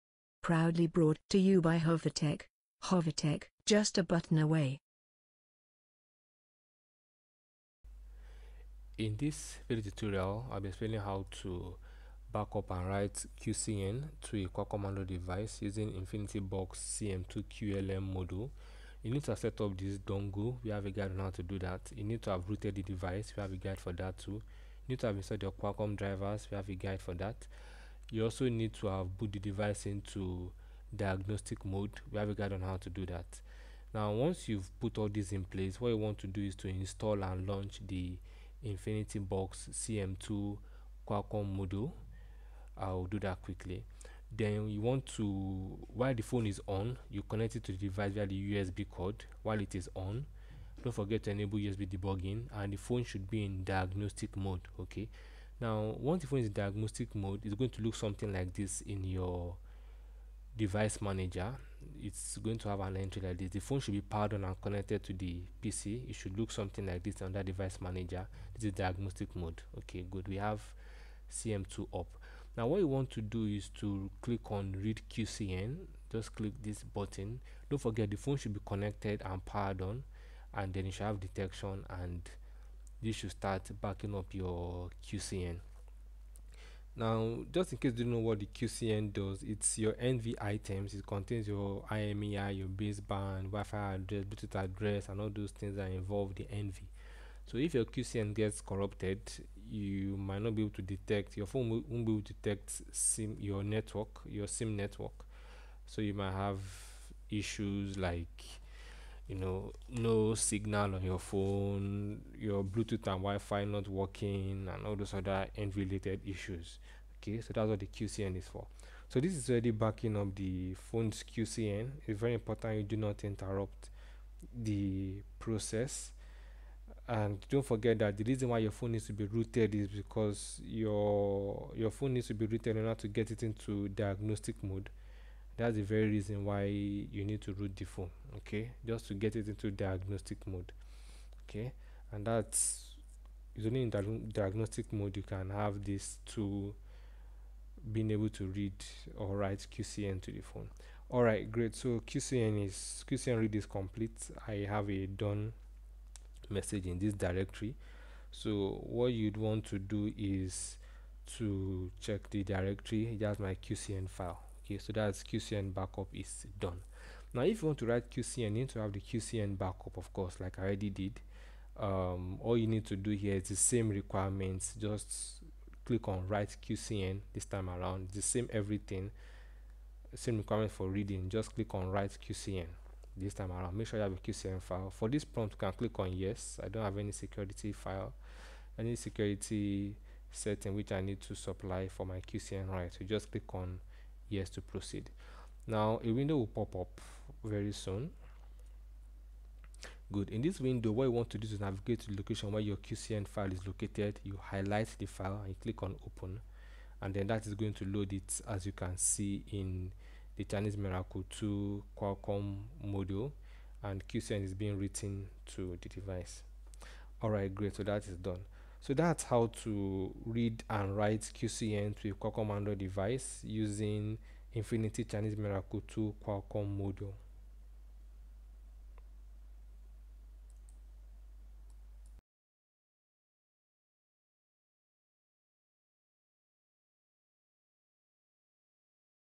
How to use Infinity CM2 to backup & write QCN to Qualcomm
This video explains how to use Infinity CM2 QLM to backup or write QCN to a Qualcomm device.

QCN is important for fixing issues like null IMEI and network problems. Ensure to only flash a compatible QCN.

00:08
In this video tutorial, I'll be explaining how to backup and write QCN to a Qualcomm Android device

00:15
using infinity box CM 2 QLM module. You
need to have setup this dongle, we have a

00:20
guide on how to do that. You need to have
rooted the device. We have a guide for that

00:24
too. You need to have installed your Qualcomm drivers, we have a
guide for that. You also need to have boot

00:29
the device into Diagnostic mode, we have a guide on how to do that.

00:33
Now, once you've put all these in place,
what you'll want to do is to install and

00:37
launch the Infinity box CM 2 Qualcomm
module. I'll do that quickly. Then You'll want

00:44
to (while the phone is on) connect the  device to PC via the USB cord (while it is on)

00:50
don't forget to enable USB debugging and
the phone should be in diagnostic mode

00:54
ok? Now, once the phone is in diagnostic mode, its going to look something like this in

00:58
your device manager, it's going to have an entry like this. The phone should be powered on

01:04
and connected to the PC. It should look
something like this under device

01:07
manager. This is diagnostic mode ok? Good we have cm2 up. Now, what you'll want to do is to click

01:14
on Read QCN. Just click this button. Don't forget the phone should be

01:18
connected and powered on and then you
should have detection and this should

01:23
start backing up your QCN. Now, just in case you don't know what the QCN does,

01:29
it's your NV items. It contains your IMEI, Baseband, Wi-Fi & Bluetooth address

01:34
and all those things that involve the NV.
So if your QCN gets corrupted,

01:40
your phone won't be able to detect your sim

01:44
network. So you might have issues like no signal on your phone, your

01:52
Bluetooth and Wi-Fi not working and all
those other NV related issues

01:56
ok? So that's what the QCN is for. So this is ready backing up the phone's QCN its very

02:02
important you do not interrupt the process and don't forget that the reason why

02:08
your phone needs to be rooted is because
your phone needs to be rooted in order

02:12
to get it into diagnostic mode. That is the very reason why you need to

02:17
root the phone okay? Just to get it
into diagnostic

02:20
mode okay? Its only in diagnostic mode you can have this tool

02:27
being able to read or write QCN to the phone. Alright! Great! So QCN read

02:33
is complete. I have a done message in
this directory so what you'd want to do

02:39
is to check the directory, that's my QCN file here. So QCN backup is

02:46
done. Now, if you want to write QCN, you'll need to have the QCN backup of course

02:50
like I already did. All you need to do here is the same requirements, just click on Write

02:56
QCN this time around. Its the same requirement for reading just click on

03:01
Write QCN this time around. Make sure you have the QCN file. For this prompt, you can

03:05
click on yes, I don't have any security
file or setting which I need

03:11
to supply for my QCN write. You just click
on yes to proceed. Now, a window will pop up

03:16
very soon. Good! in this Window, what you'll want to do is to navigate to the location where your QCN

03:23
file is located. You highlight the file
and click on Open

03:26
and then that is going to load it as you
can see in the Chinese miracle II

03:31
Qualcomm module and QCN is being written to the device. Alright! Great! So that

03:37
is done. That's how to read and write
QCN to a Qualcomm Android device using

03:44
infinity Chinese Miracle 2 Qualcomm module
Hire Or Consult Us